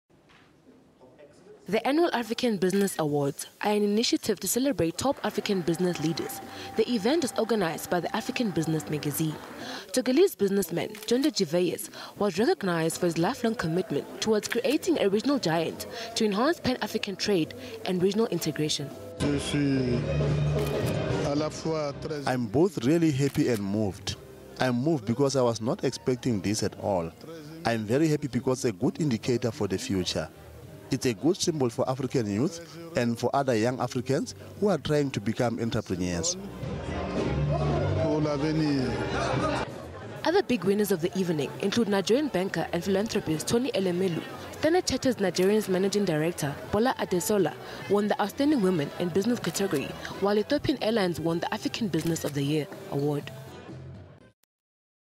Gervais Koffi Djondo was honoured at the 2013 annual African Business awards
Top African business leaders were honoured at the UN General Assembly ahead of a world summit this week. Gervais Koffi Djondo, the co-founder of one of Africa's fastest growing financial institutions Ecobank group, was honoured with a Lifetime Achievement award at the 2013 annual African Business awards, held in New York